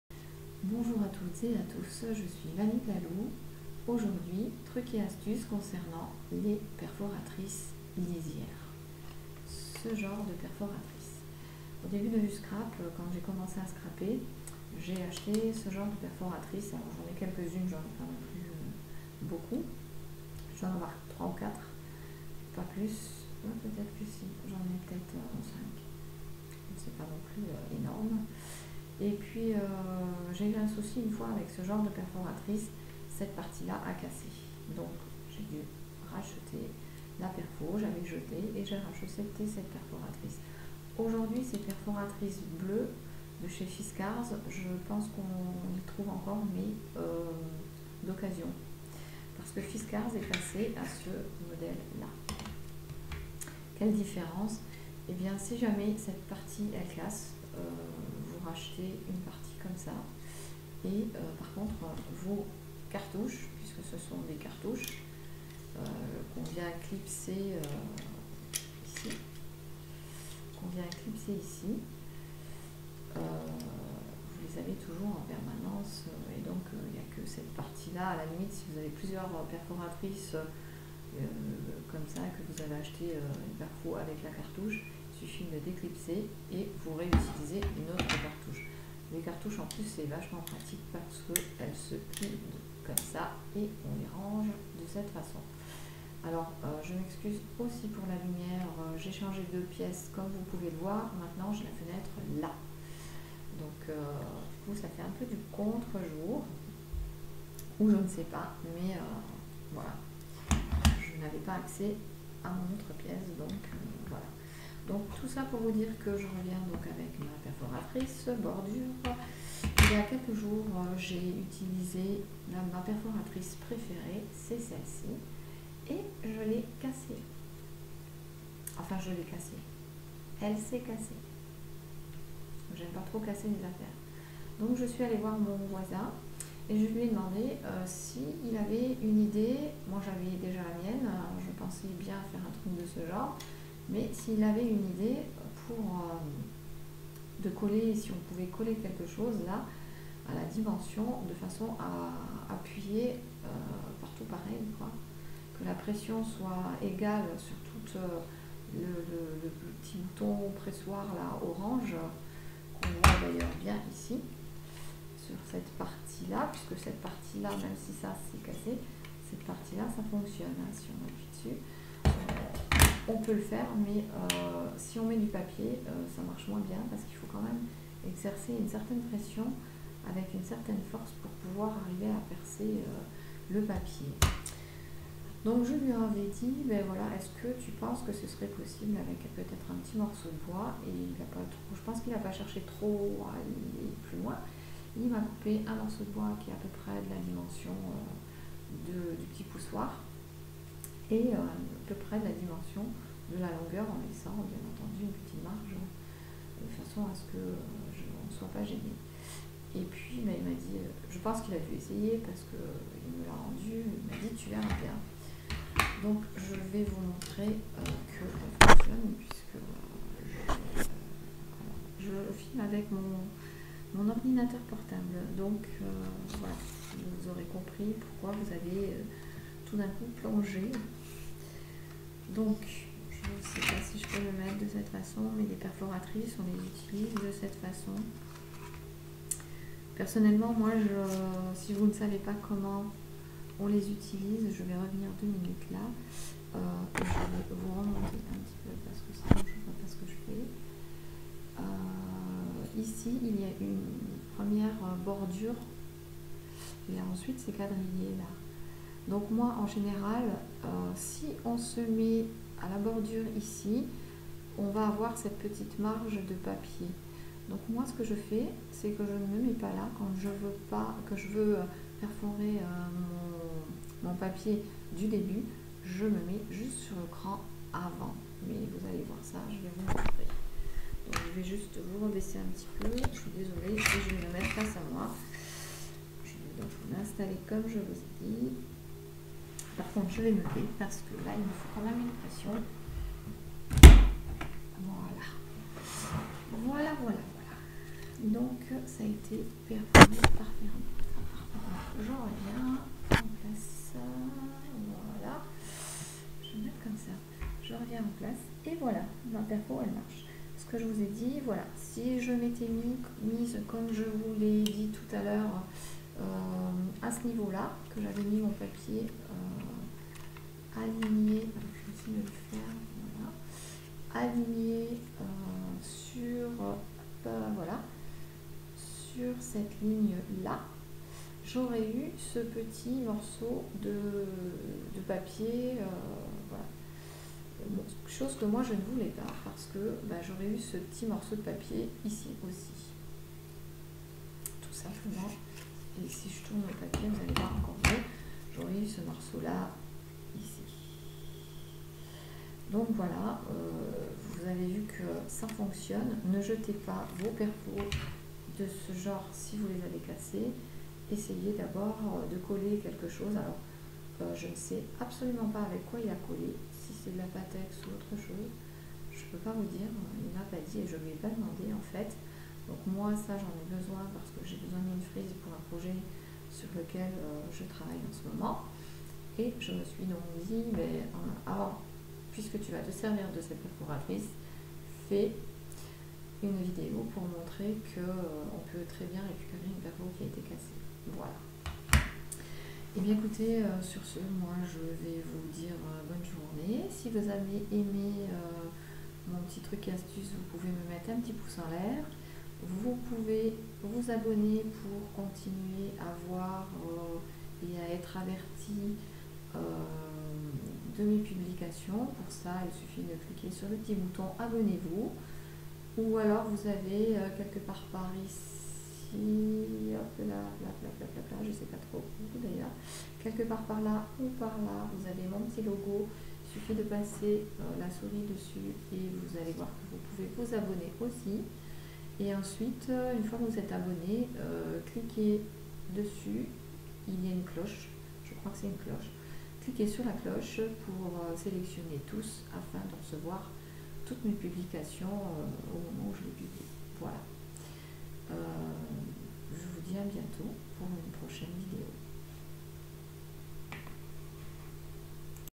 Comment récupérer une perforatrice bordure fiskars cassée
Bonjour à toutes et à tous, dans cette vidéo je vous montre comment ma perfo bordure préférée et de nouveau utilisable. Alors avant de jeter les vôtres essayez.
Si vous avez aimé laissez moi un pouce en l'air.